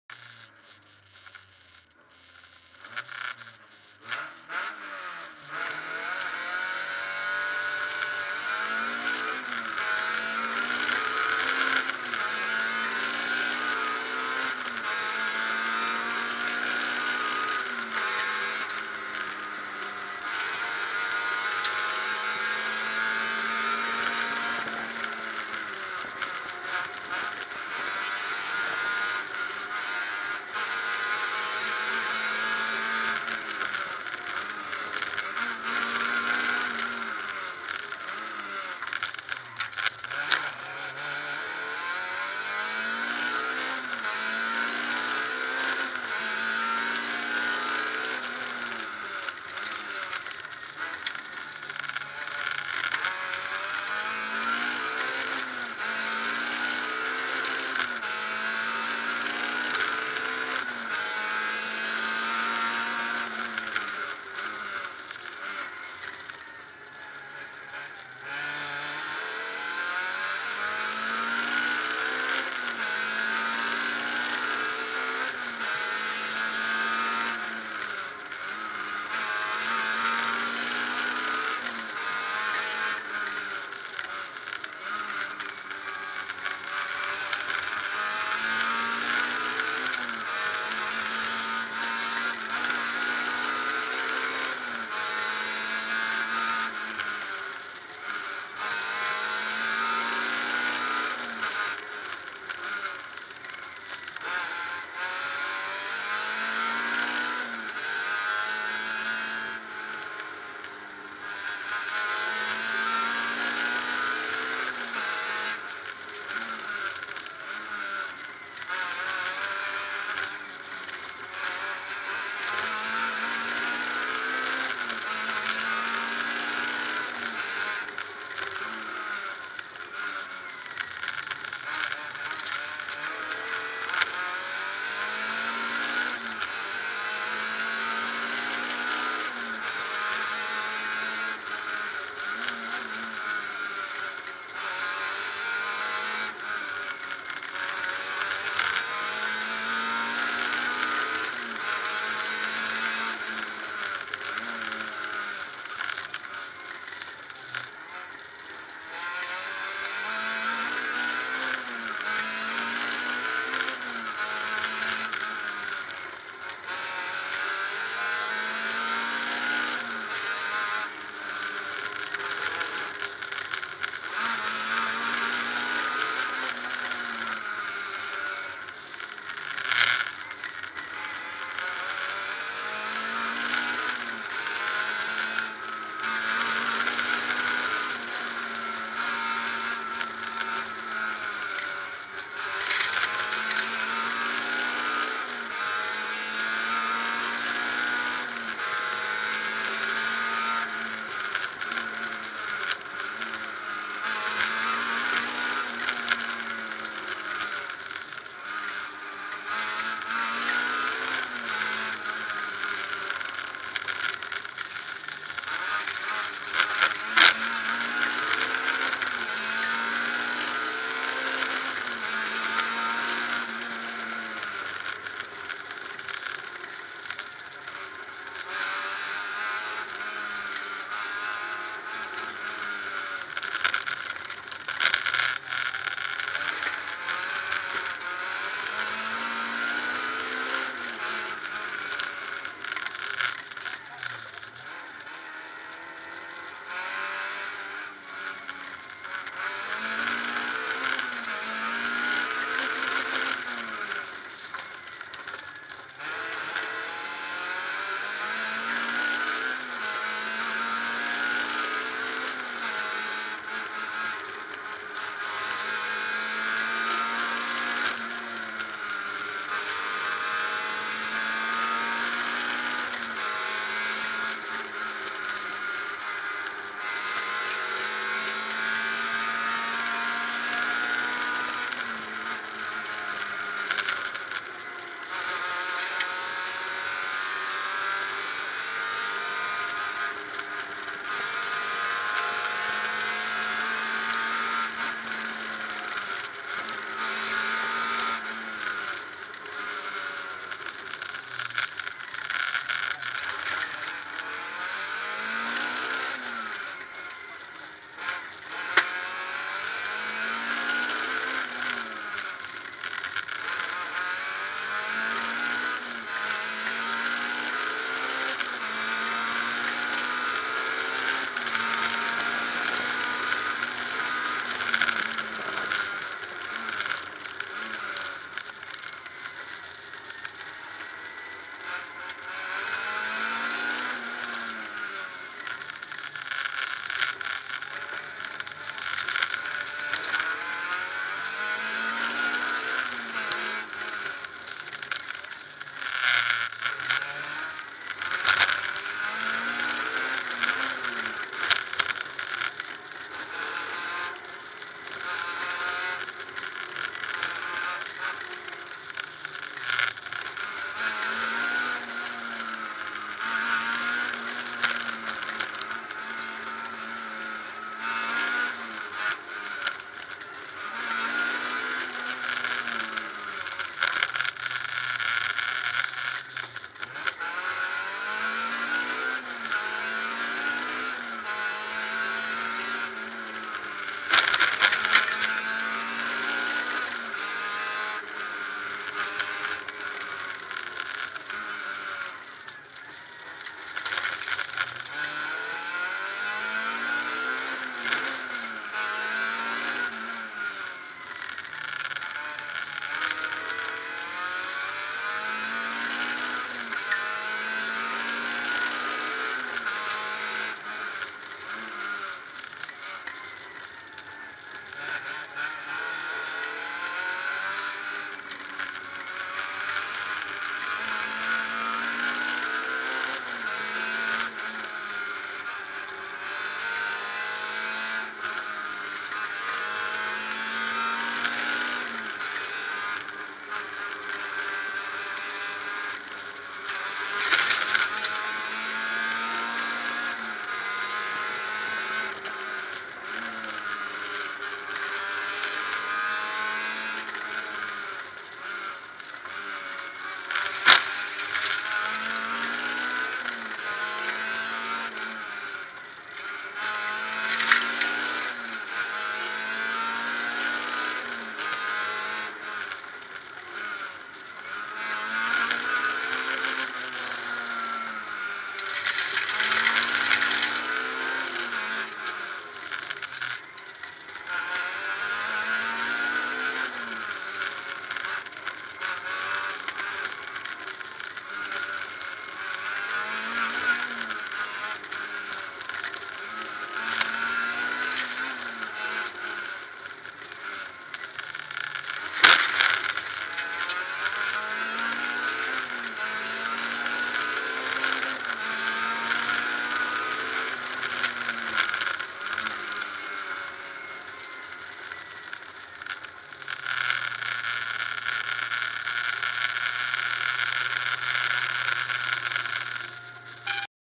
RALLYE DES BORNES 2018 ES8 pers jussy
caméra embarquée PEUGEOT 306 A7 n°62
SALTAPES Sébastien / ROUX Emmanuel
54/112 3ème groupe A7